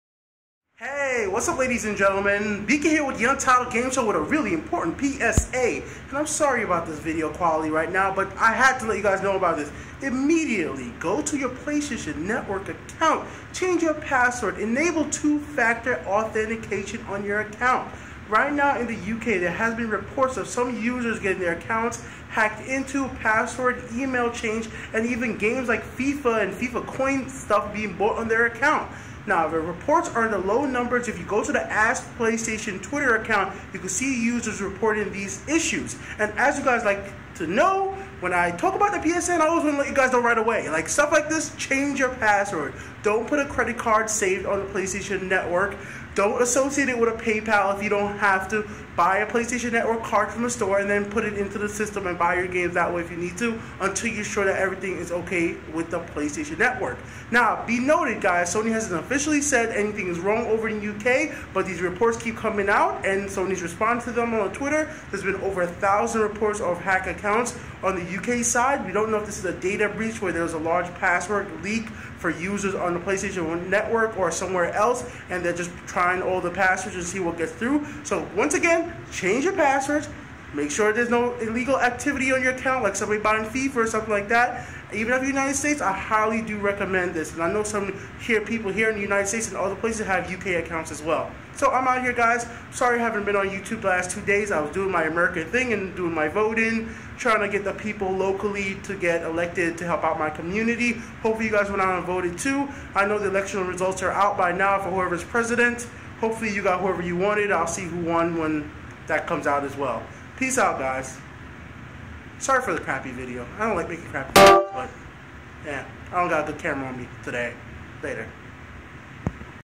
PSA: Update PlayStation Network Account Info. PSN Potentially Hacked?
Sorry quality but this important go Sony PlayStation network account and check your transaction activity and update your password just to be safe.

Why risk it?